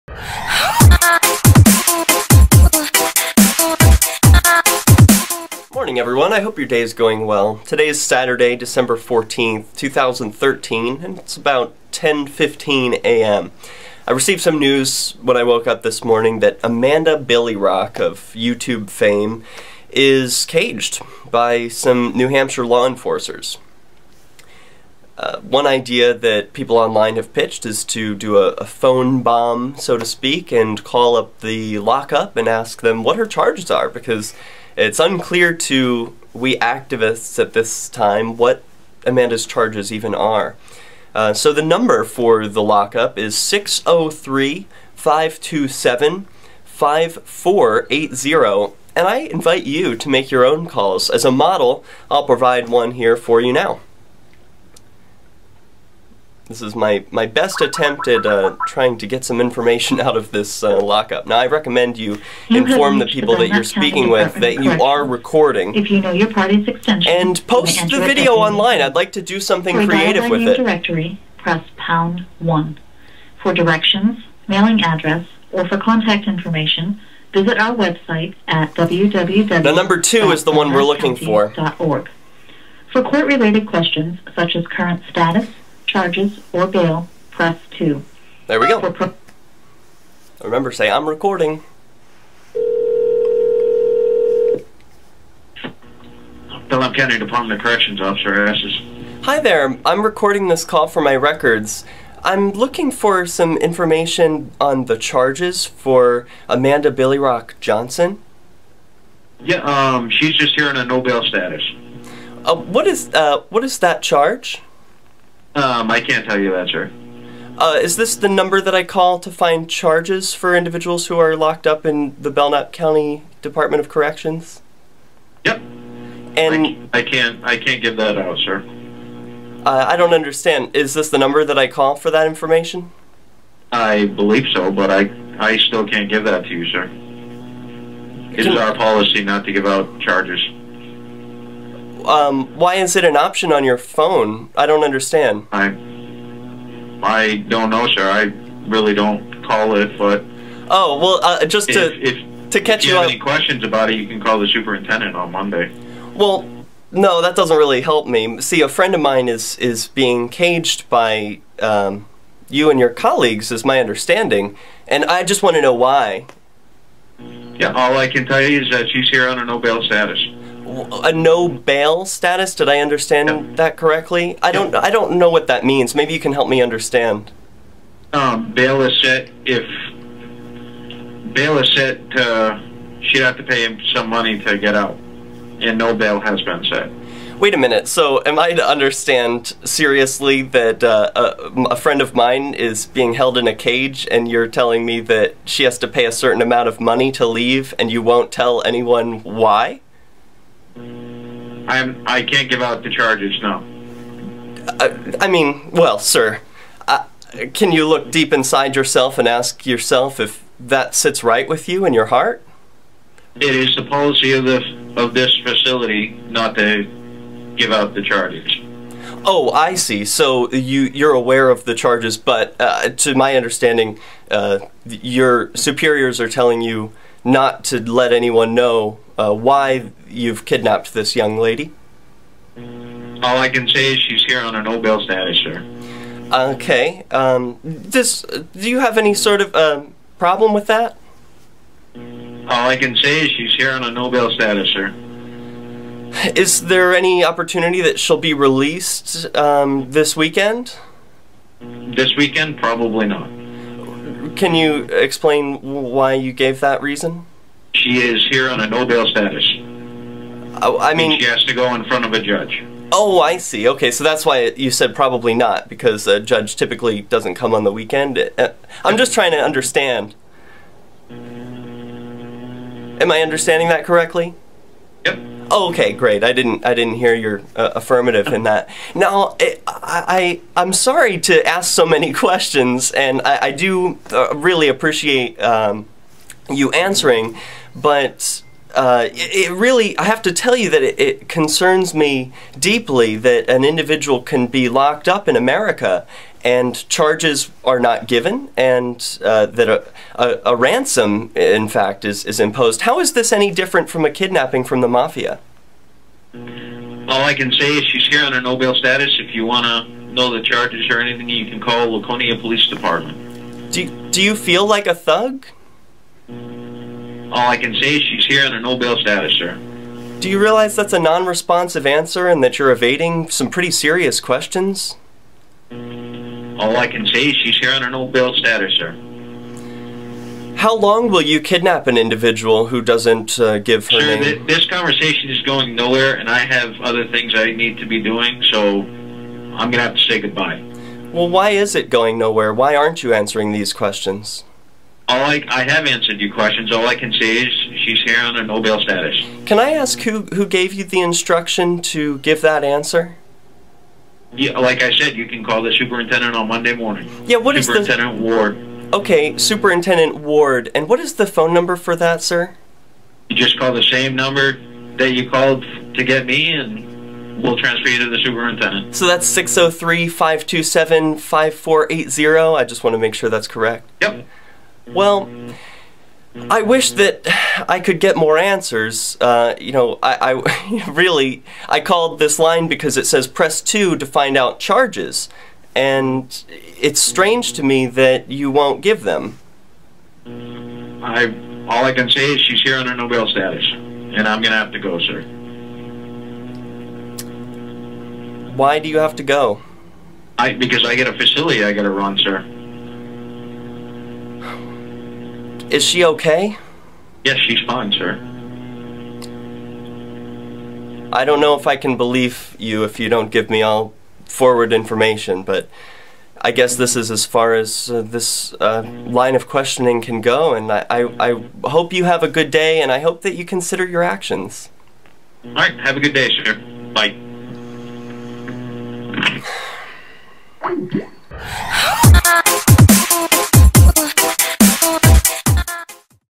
Amanda BillyRock CAGED! Reason UNKNOWN??
The cops have kidnapped a peace activist in New Hampshire aren't giving any answers as to why. Does this sit right with you?